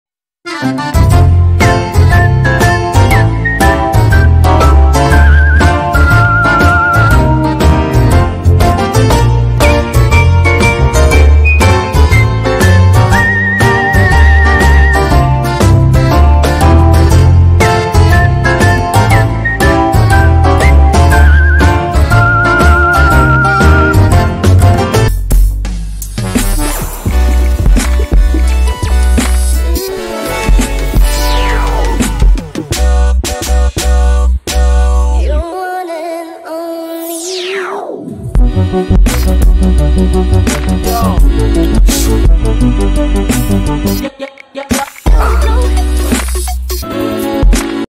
cristal slipped on floor😂😂🤣🤣 frenchbulldog lahsa apso funnydogs videos
searching for happines . . crystal rhe frenchbulldog and happy the lahsa apso enjoying by playing with each other. . .